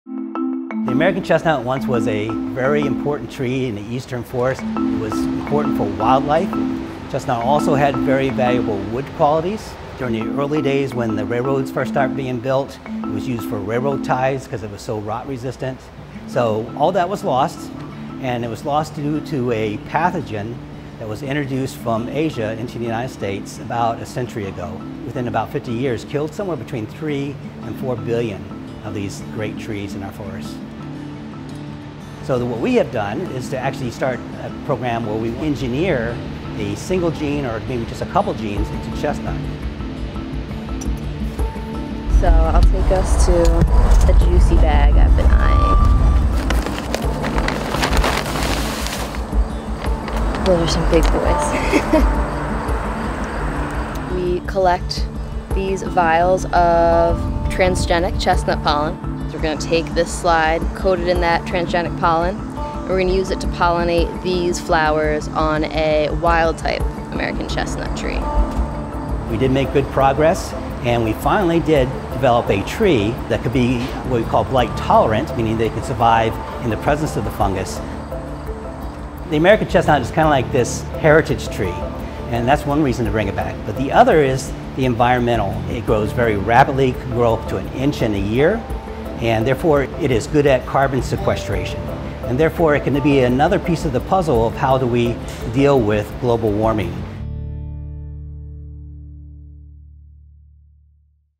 Gene editing could save the threatened American chestnut
Chestnut enthusiasts have been trying for decades to revive a threatened tree species that was once celebrated from Maine to Mississippi. Now, scientists in Syracuse, N.Y., say they’ve created a genetically modified version of the American chestnut that can withstand damage from an exotic fungus.